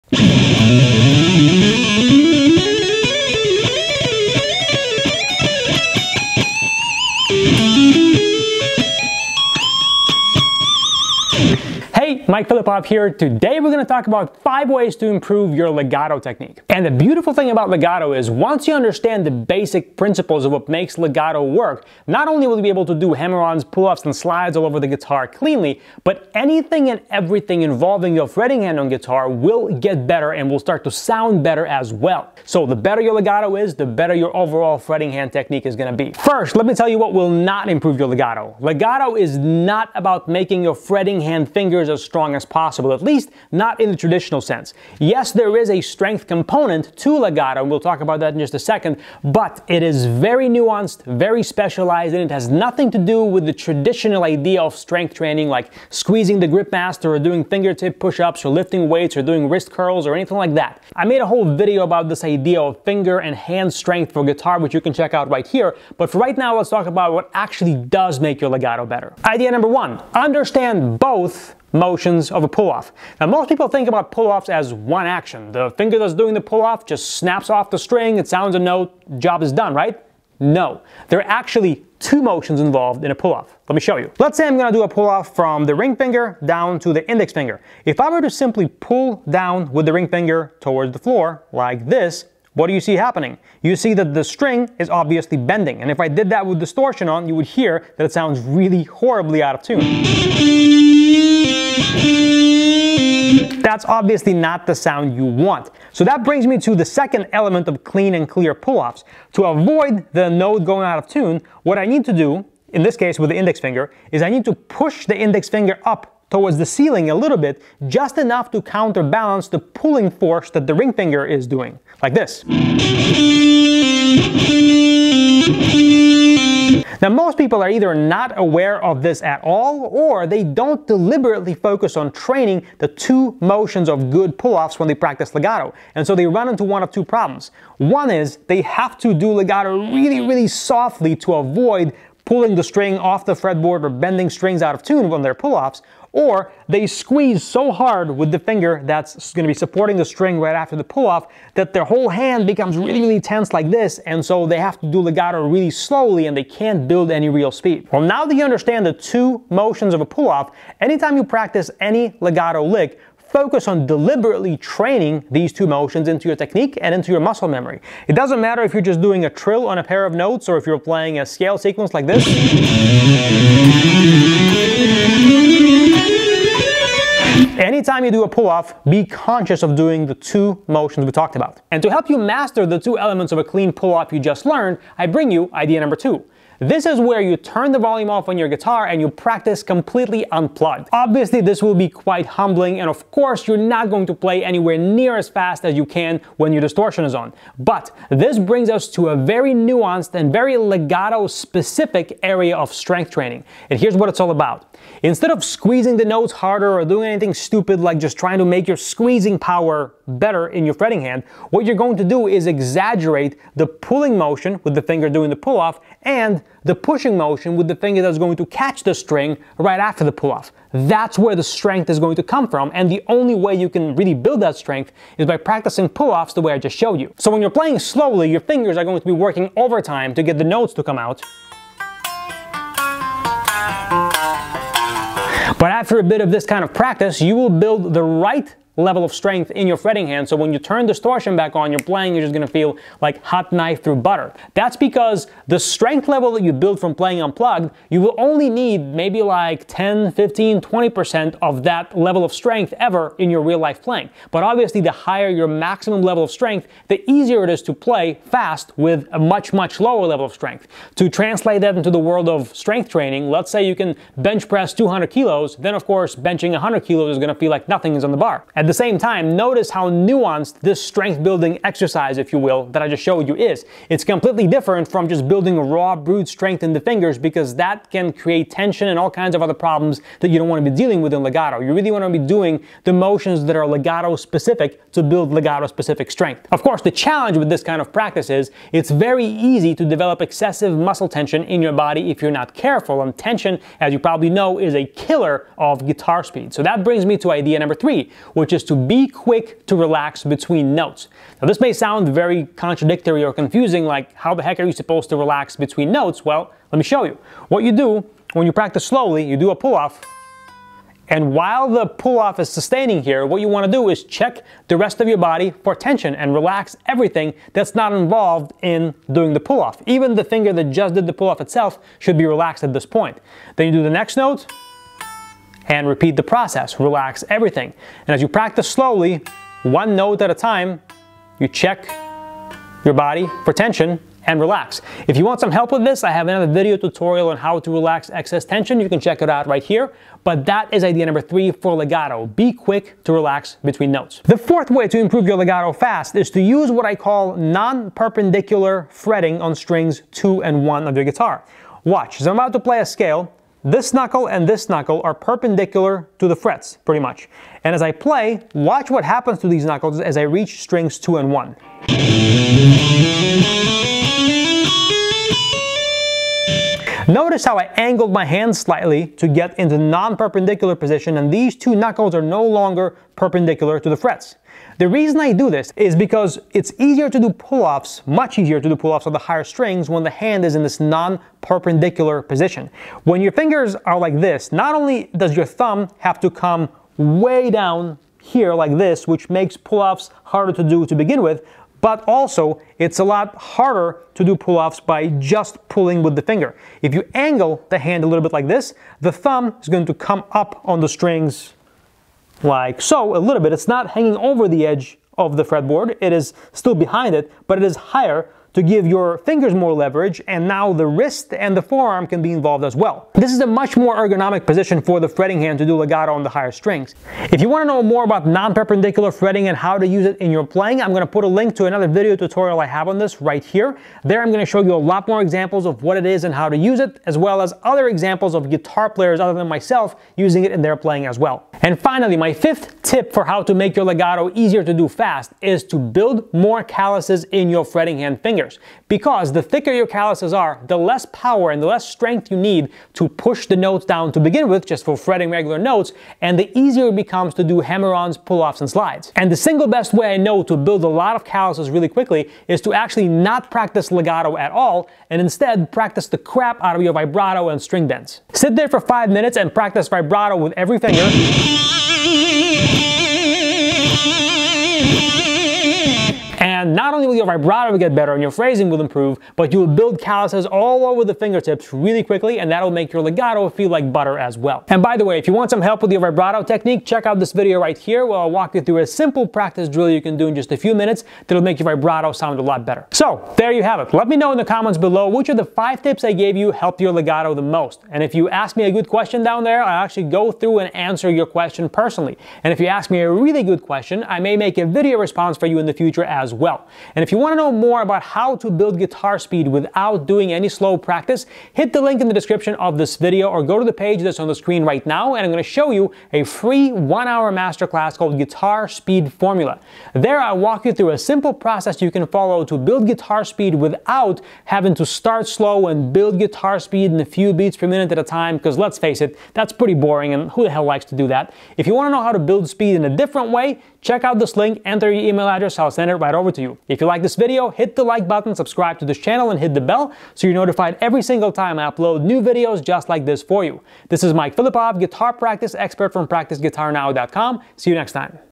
5 Easy Ways To Do Legato Cleaner & Faster
- free video master class on building guitar speed without slow practice.

This video is about 5 ways to do legato cleaner and faster on guitar.

0:00 Start
0:36 - What does NOT improve legato
1:09 - Idea #1
3:05 - Idea #2
4:56 - Idea #3
5:44 - Idea #4
7:11 - Idea #5